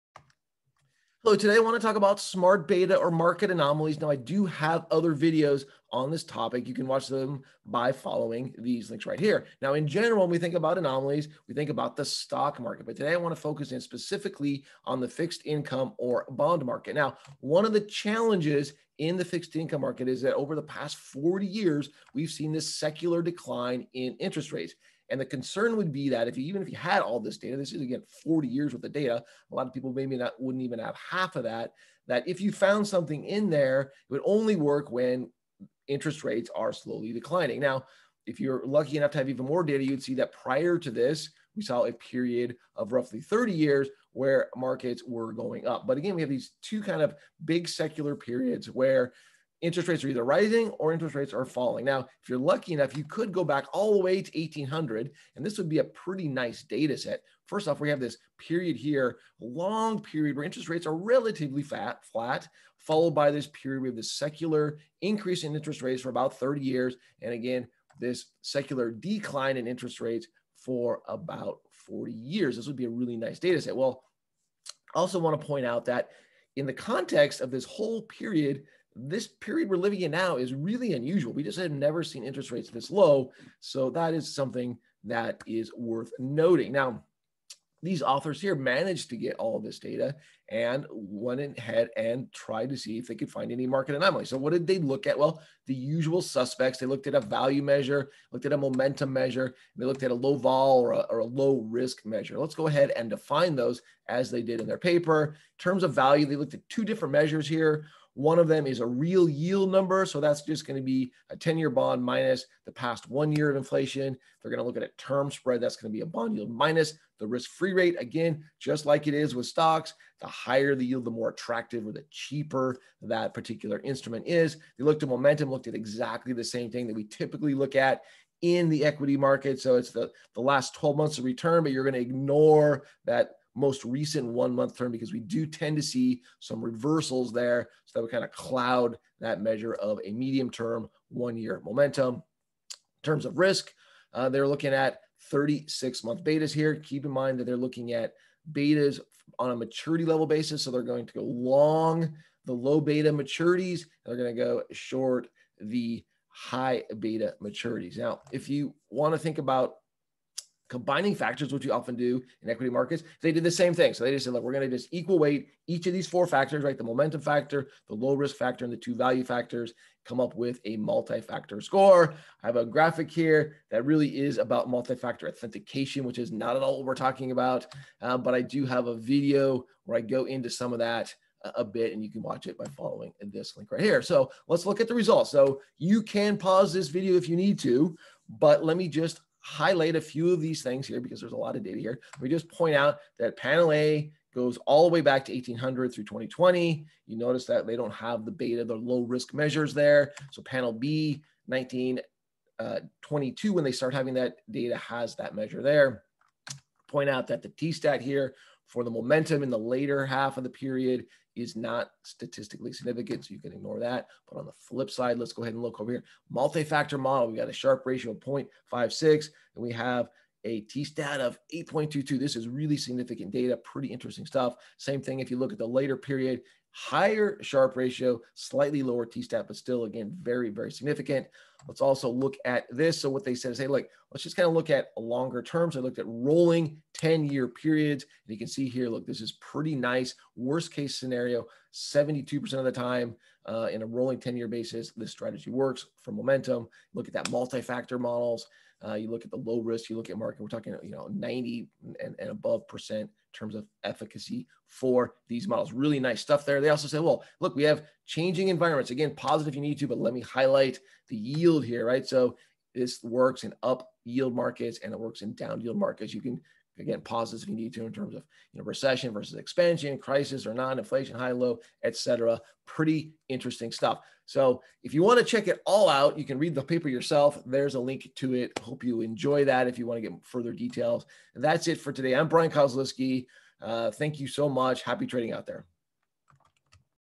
Smart Beta Fixed Income Markets
Anomalies in equitymarkets are well documented. However, the same cannot be said for bondmarkets. A major problem is that we are in the midst of an 40 year secular decline in interestrates. Even if you have access to all this data, you might discover an anomaly that only works in a declining interest rate environment. Guido Baltussen, Martin Martens and Olaf Penninga examined value, momentum and low risk smartbeta strategies in the fixedincome market using international data going back to 1800 and find that anomalies exist for bonds too. A multi-factor approach produces high Sharpe ratios and very significant t-stats.